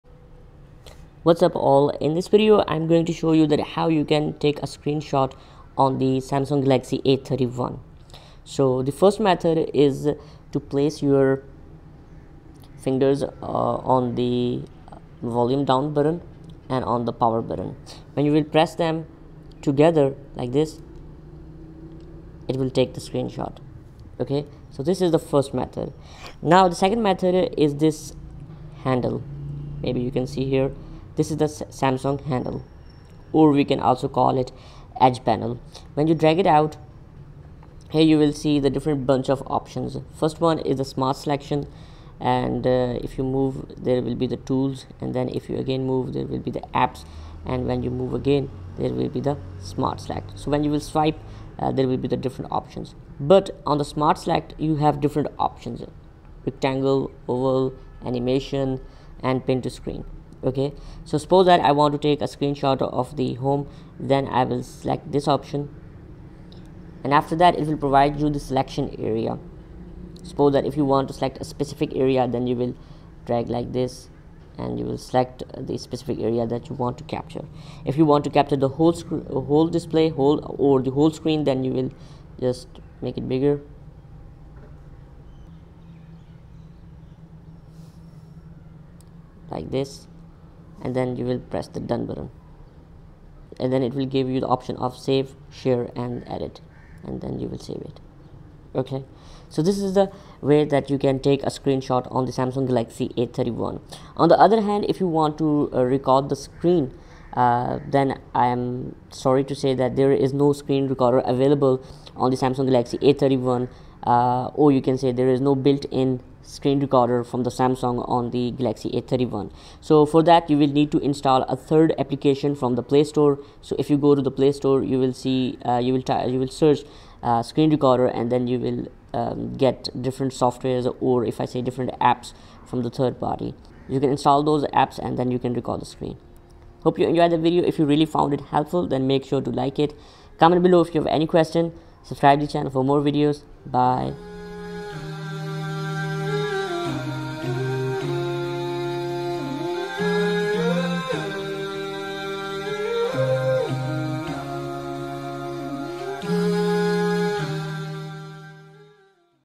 How To Take Screen Shot & Record Screen On Samsung Galaxy A31
How To Take Screen Shot On Samsung Galaxy A31?
How To Record Screen On Samsung Galaxy A31?

 I am going to quickly answer both questions in this video and show to method. Watch till end for full information.